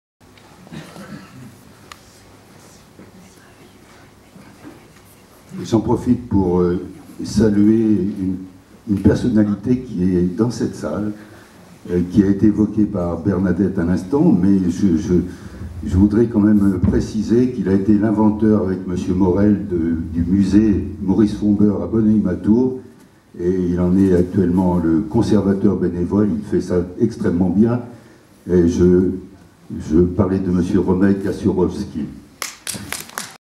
Fombeure
on a parlé de Maurice Fombeure à Chatellerault